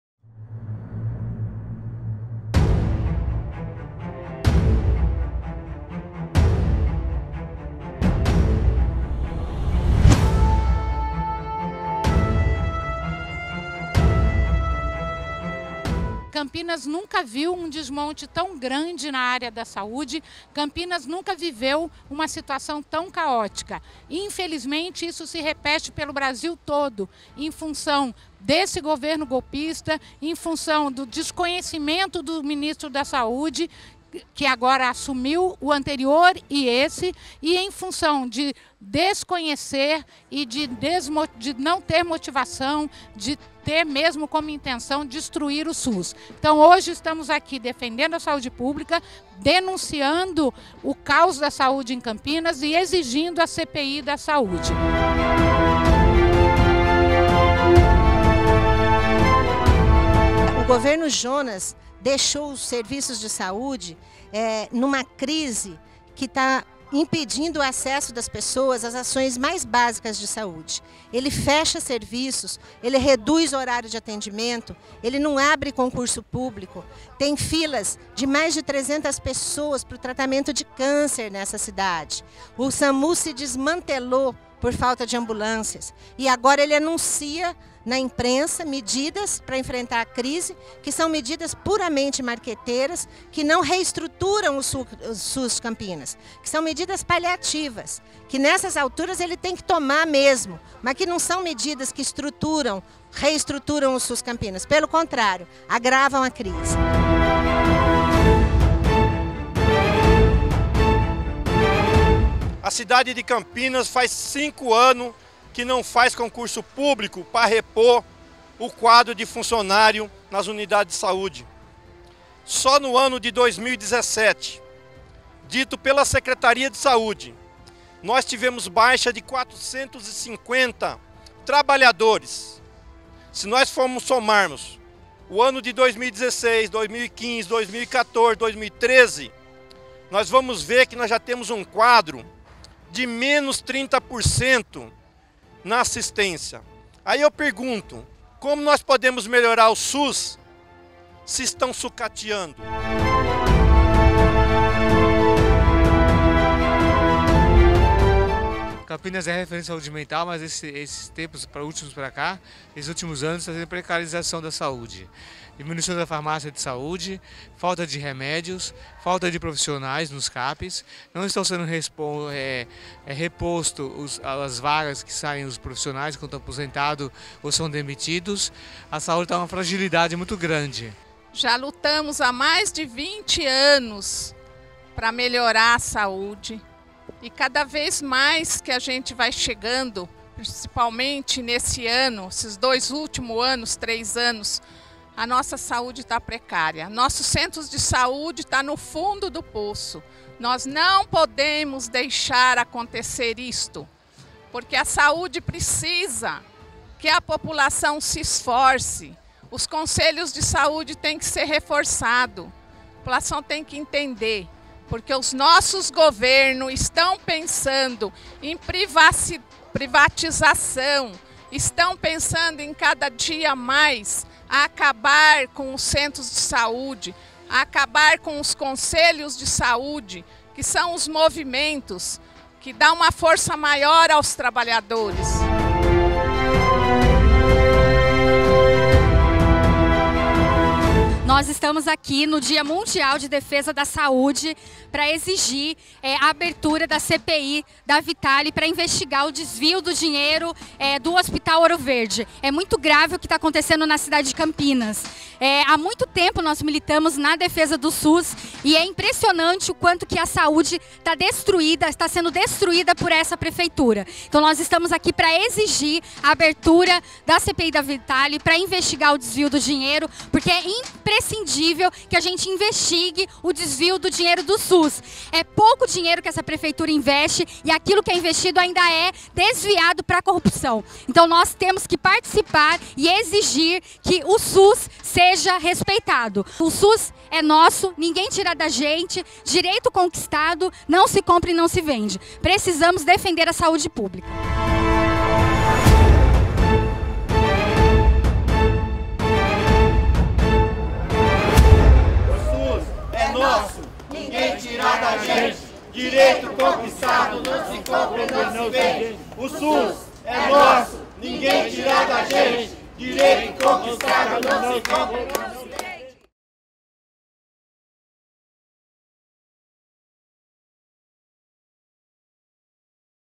Manifestação contra o desmonte da Saúde
O Sistema Único de Saude(SUS) no Brasil é uma conquista da luta de amplos setores da nossa saciedade que veem na sua existência, no seu fortalecimento e na sua consolidação a possibilidade de garantir ao povo brasileiro a igualdade em relação a um direito de cidadania que é a manutenção da própria vida.
Em Campinas o ataque a ataque ao SUS intensificando pela pior gestão municipal na área   da saúde que já tivemos. Serviços parados por falta de materiais e equipamentos,SAMU em crise por falta de ambulâncias, diminuição do quadro de pessoal, falta de medicamento, trabalhadores desmotivados sobrecarregados e sem espaço democrático de debates.
Venha lutar pelo direito à Saúde! Em defesa do SUS 100% público de qualidade e democrático!
Contra a péssima gestão do Governo Jonas e do Secretário Cármino!